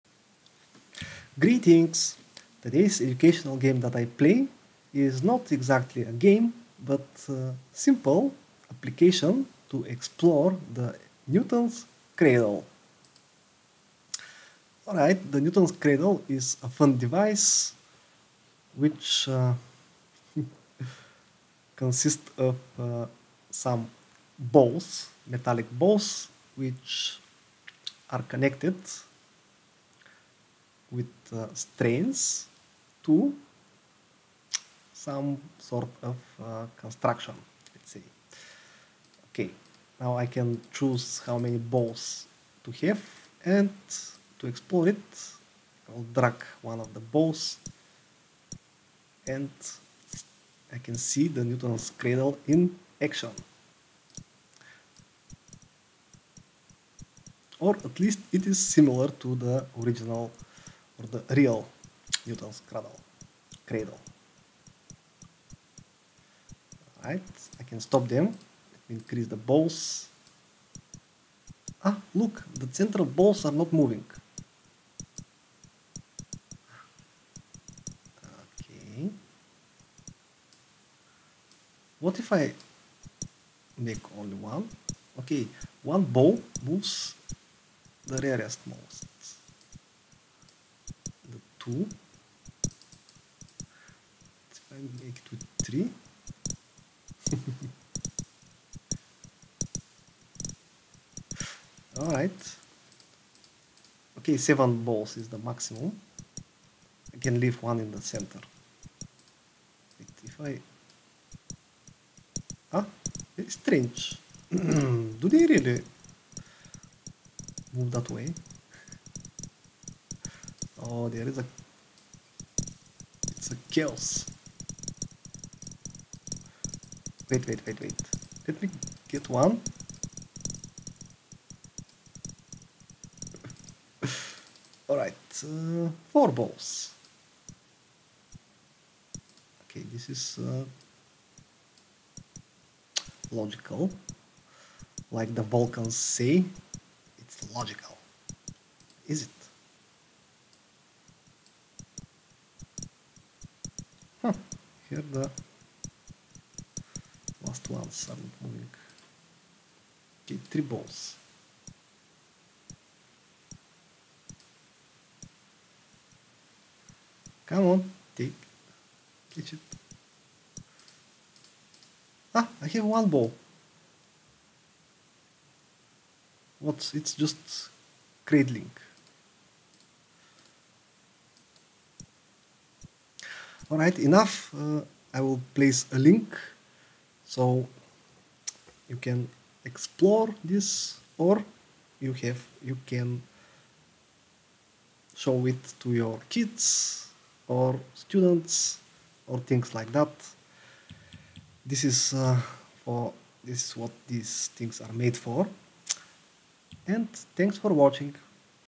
Newton's Cradle - Interactive Physics Lesson 🚀🎥🎓
Fun educational video, where I try to explore the Newton's Cradle.